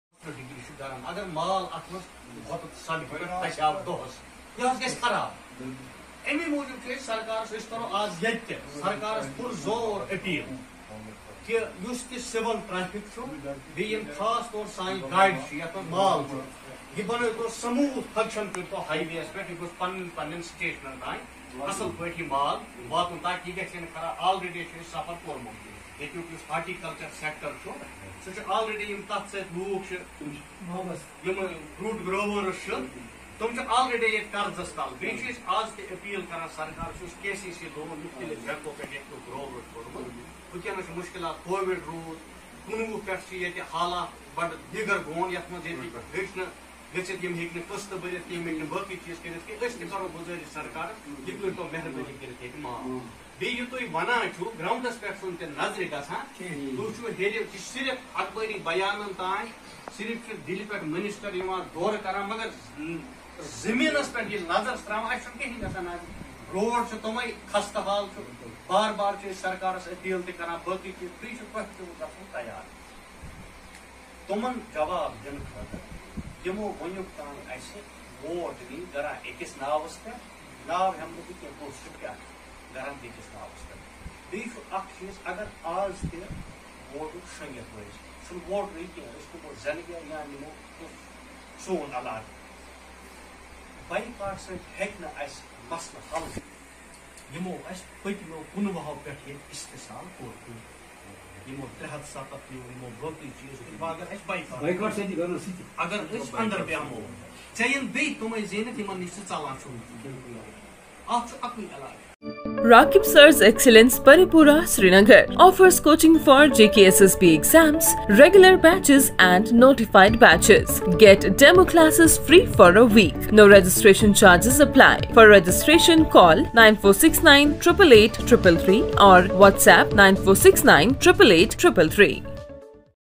Altaf Malik Urges LG Administration To Ensure Smooth Civil Traffic Movement on Highway
Altaf Malik Urges LG Administration To Ensure Smooth Civil Traffic Movement on Highway espicially kashmiri Apple Laden Trucks Held workers meeting at wadoora